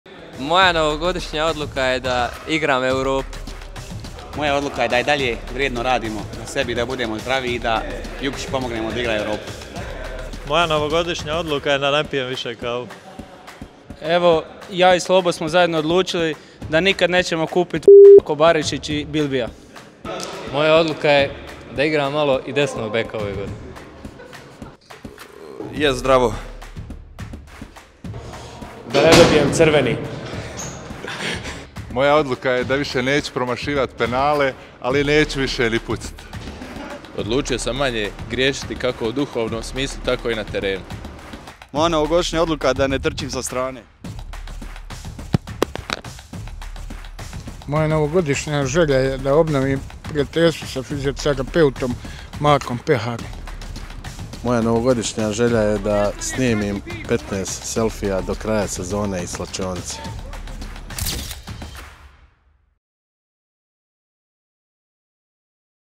Novogodišnje želje i odluke igrača HŠK Zrinjski Mostar
Koje su to novogodišnje odluke i želje naših igrača i stožera saznajte u ovom videu!